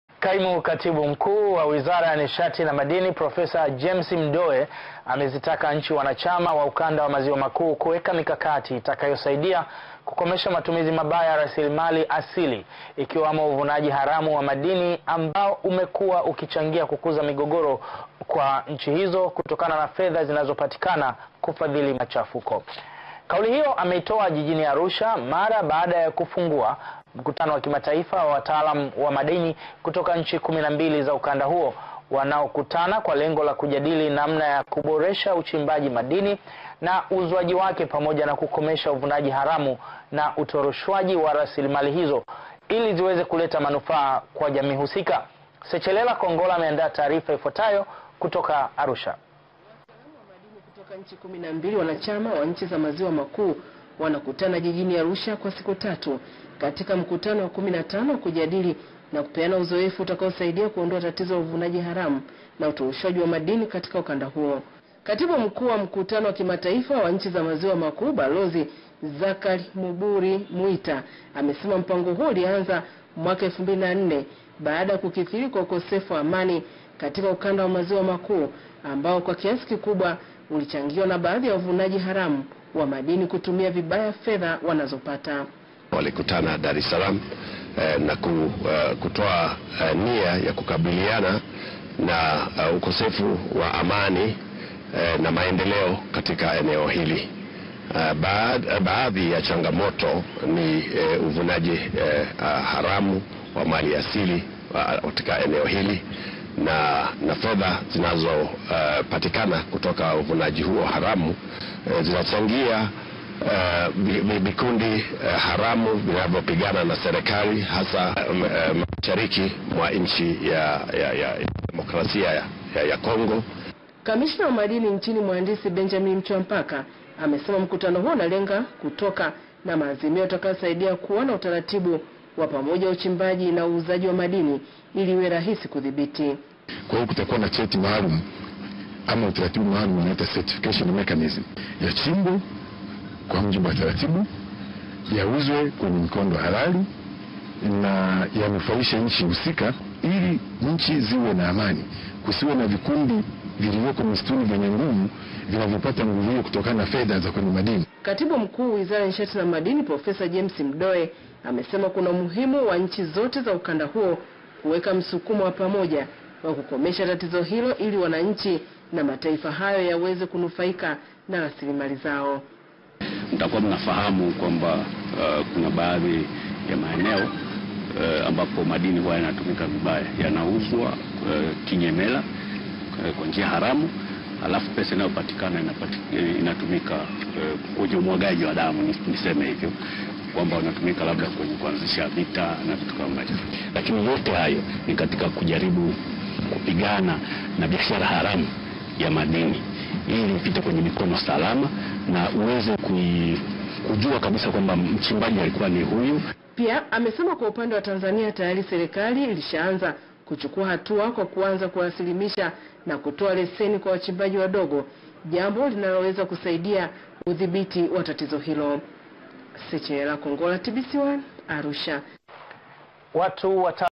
Ulinzi Wa Rasirimali Za Nchi
Kaimu katibu mkuu wizara ya nishati ya madini amezitaka nchini jirani kuweka mkakati utakaosaidia kukomesha wizi wa rasirimali za nchi husika